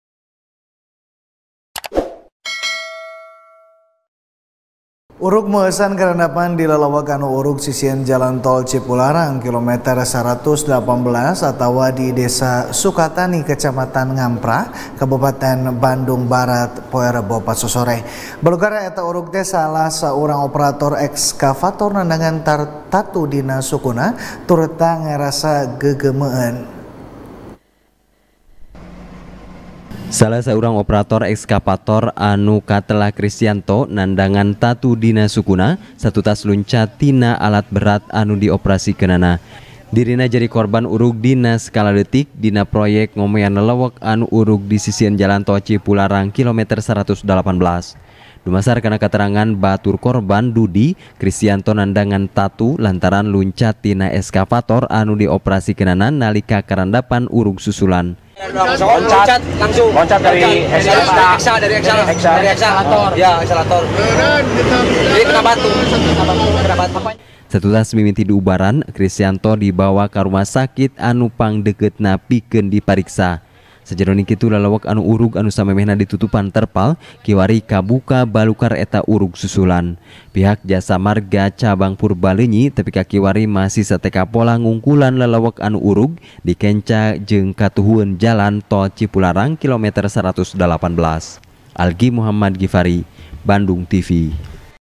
Urug Susulan Di Tol Purbaleunyi, Salah Saurang Operator Eksavator Nandangan Tatu | TP | BANDUNGTV
TP | BANDUNGTV | DERI FIRMAN | Urug Meueusan Karandapan Di Lelewek Anu Urug Sisieun Jalan Tol Cipularang Kilometer Saratus Dalapan Belas, Atawa Di Desa Sukatani, Kecamatan Ngamprah, Kabupaten Bandung Barat, Poe Rebo Pasosore. Balukar Eta Urug Teh, Salah Saurang Operator Eskavator Nandangan Tatu Dina Sukuna Turta Ngarasa Geugeumeueun.